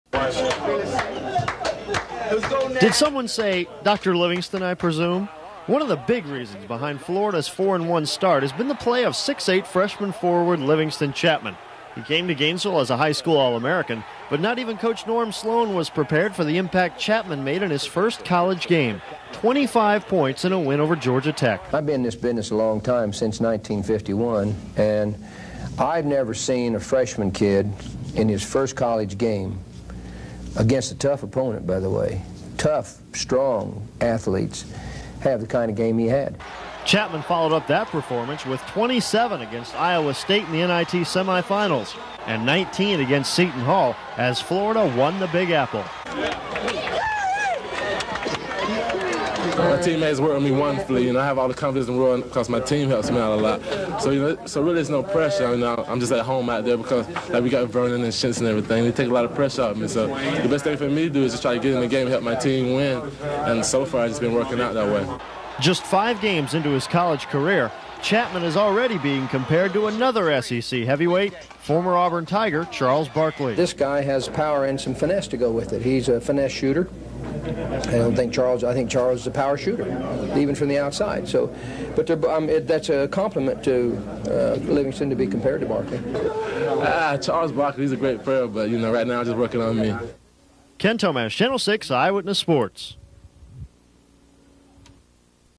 Livingston Chatman, Florida Gator Basketball, 1988
During a season in which he would be named to the SEC All-Freshman team, Florida's Livingston Chatman lived up to his Parade All-American hype. He scored 25 points in his first college game, and averaged 12.5 ppg that season. This story from WCTV-TV in Tallahassee, Fla. profiled the freshman from Lakeland.